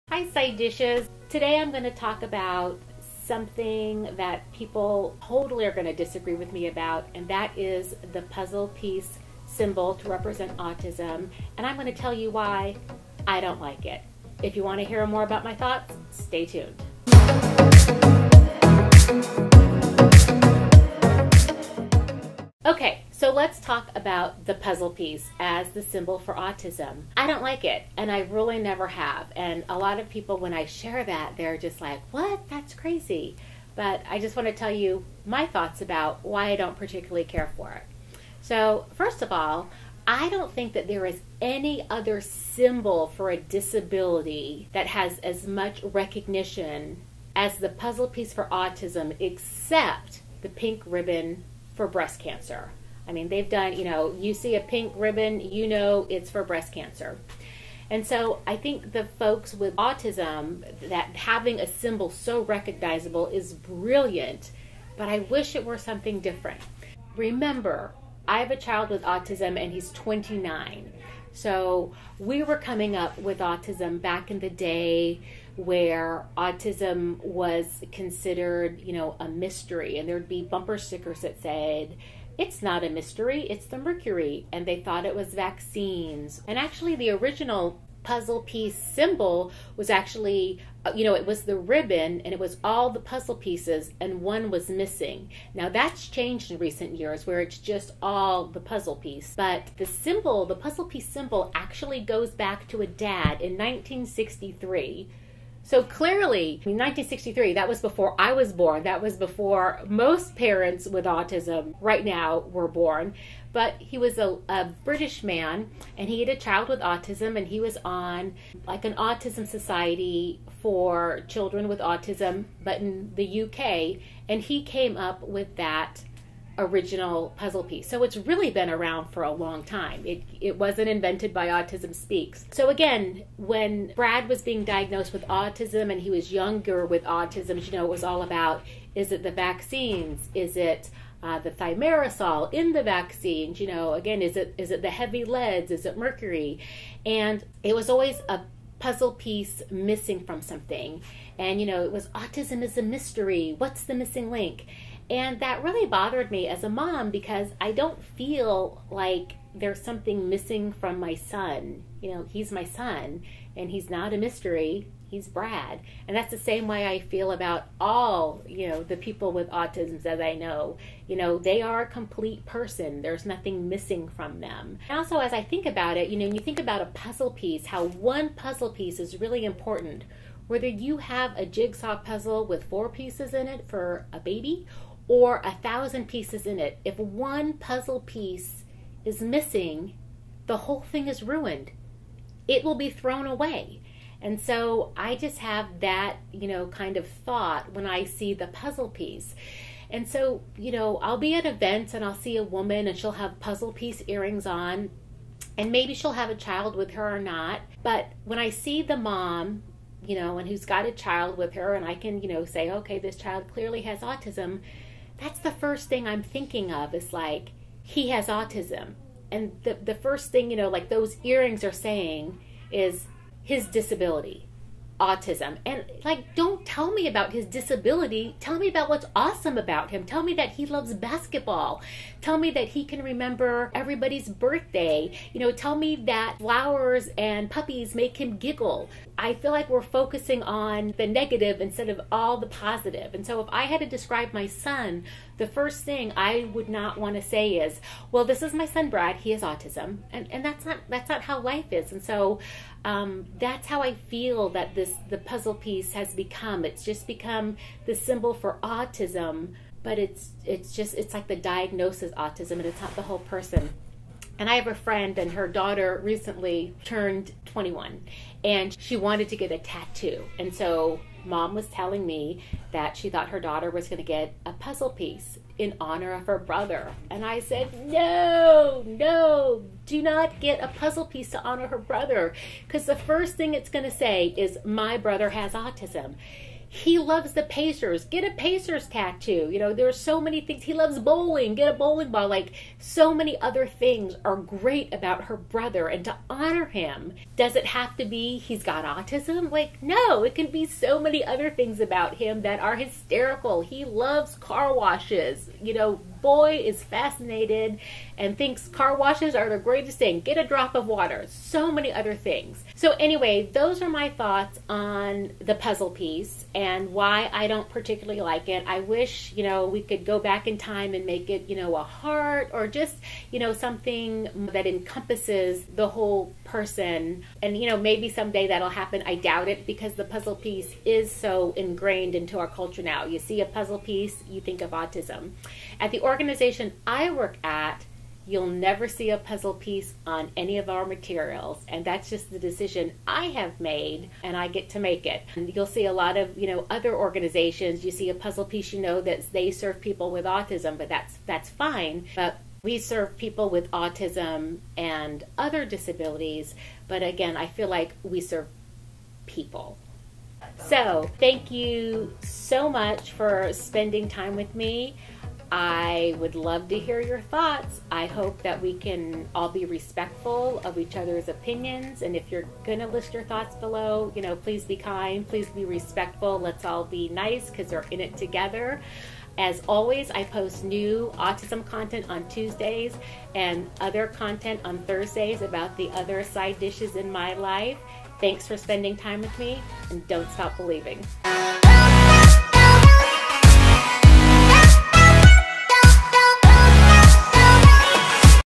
Why I HATE The PUZZLE PIECE SYMBOL For AUTISM - Mary Leonilda
Hi Side Dishes!
You might see this title and wonder, HOW in the world could I say that I hate the puzzle piece symbol as the sign for autism,  but let me explain. I have strong feelings about this but I will tell you exactly why in this video.